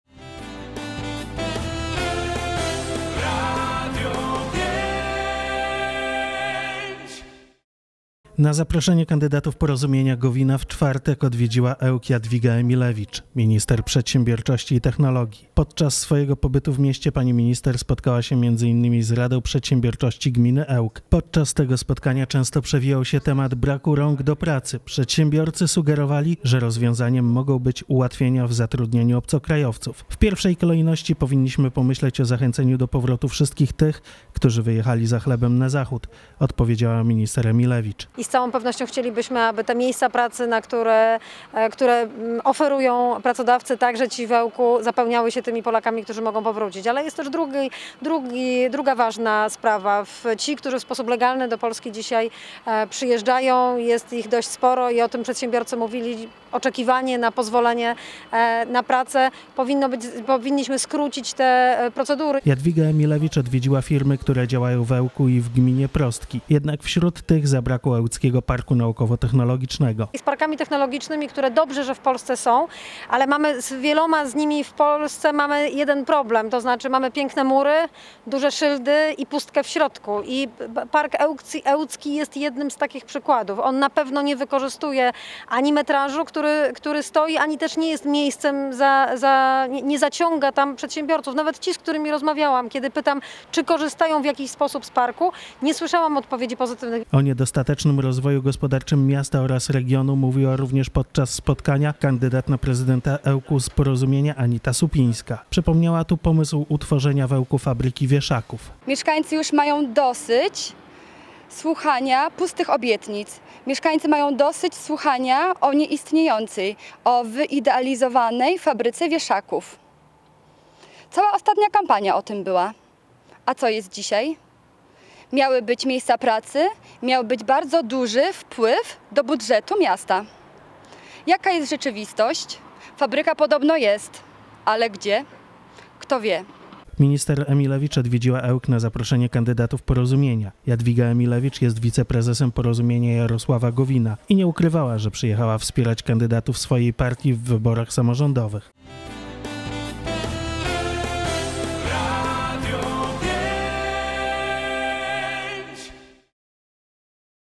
O wieszakach, Technoparku i wyborach | Radio 5
Na zaproszenie kandydatów Porozumienia Gowina, w czwartek (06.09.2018) odwiedziła Ełk Jadwiga Emilewicz - Minister Przedsiębiorczości i Technologii. Podczas swojego pobytu w mieście pani minister spotkała się między innymi z Radą Przedsiębiorczości Gminy Ełk. Podczas tego spotkania często przewijał się temat braku rąk do pracy. Przedsiębiorcy sugerowali, że rozwiązaniem mogą być ułatwienia w zatrudnianiu obcokrajowców. - W pierwszej kolejności powinniśmy pomyśleć o zachęceniu do powrotu wszystkich tych, którzy wyjechali za chlebem na zachód - odpowiadała minister Emilewicz. Jadwiga Emilewicz odwiedziła firmy, które działają w Ełku i w gminie Prostki. Jednak wśród tych zabrakło ełckiego Parku Naukowo-Technologicznego. O niedostatecznym rozwoju gospodarczym miasta oraz regionu mówiła również podczas spotkania kandydat na prezydenta Ełku z Porozumienia Anita Supińska. Przypomniała tu pomysł utworzenia w Ełku fabryki wieszaków. Minister Emilewicz odwiedziła Ełk na zaproszenie kandydatów Porozumienia. Jadwiga Emilewicz jest wiceprezesem Porozumienia Jarosława Gowina i nie ukrywała, że przyjechała wspierać kandydatów swojej partii w wyborach samorządowych.